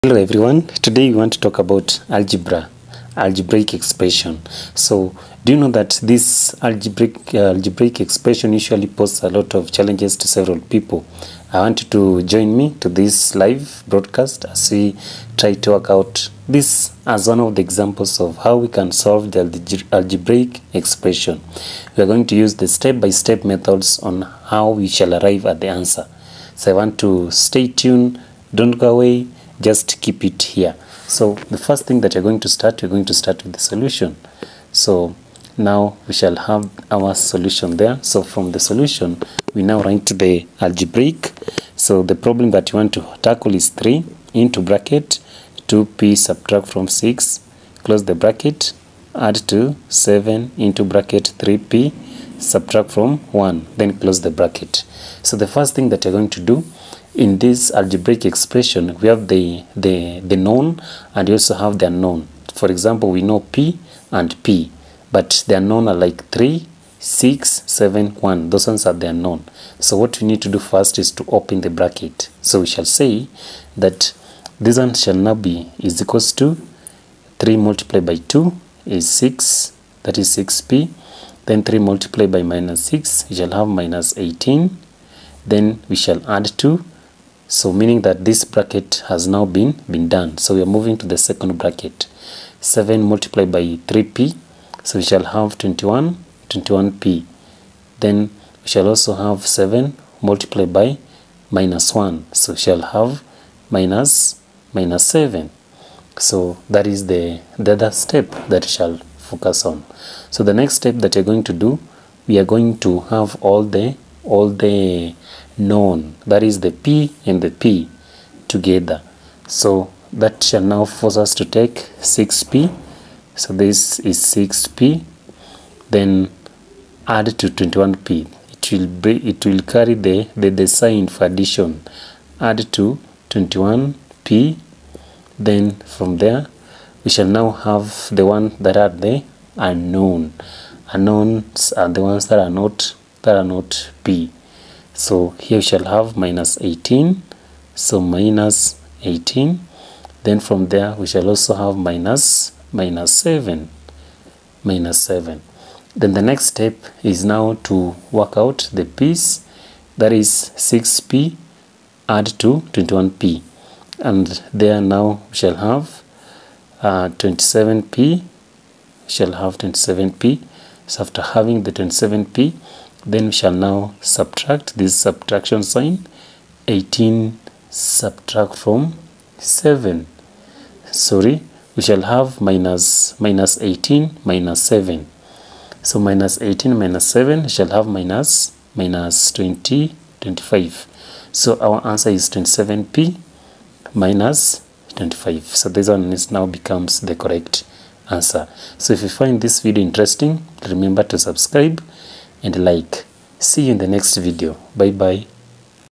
Algebraic Expressions Grade Seven Questions Simplified
How to Simplify algebraic expression for beginners.